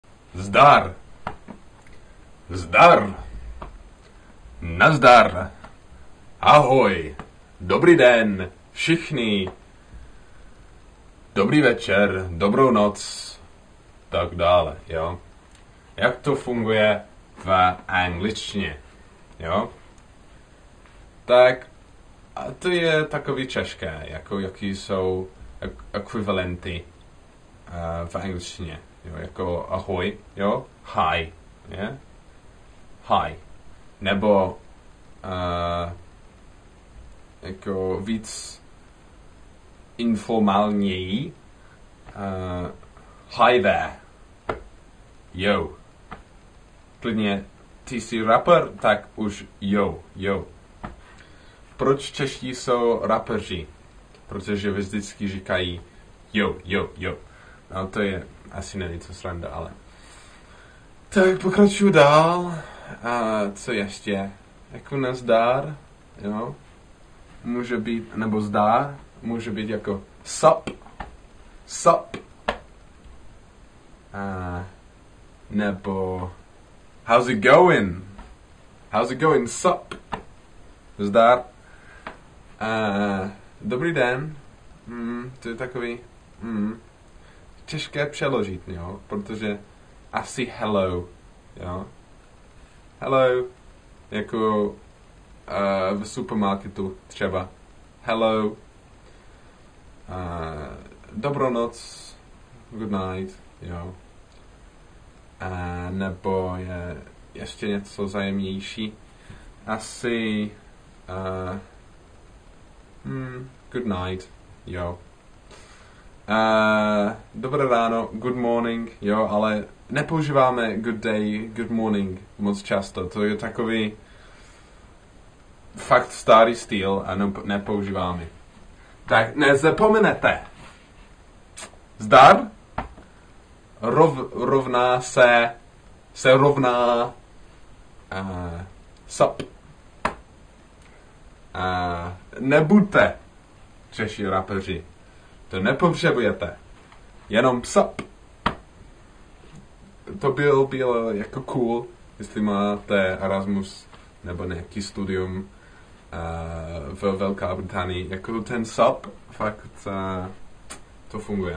Pozdravy v angličtině (s rodilým mluvčím)
Italki is the best way to find teachers of English. Here's $5 to help kickstart your journey. Video o anglictine! Nazdar, ahoj, dobry den. Jak to funguje v anglictine?